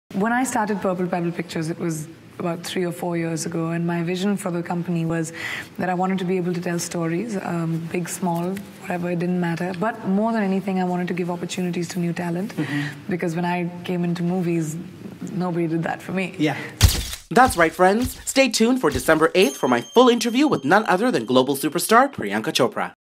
PROMO #2: Priyanka Chopra Talks About Becoming A Producer | DATE WITH DANIEL
Watch my FULL interview with global superstar Priyanka Chopra at the International Film Festival of South Asia as she talks about the journey to becoming a producer with her new production company Purple Pebble Pictures. She also reveals why its important for her to support new talent and become a storyteller.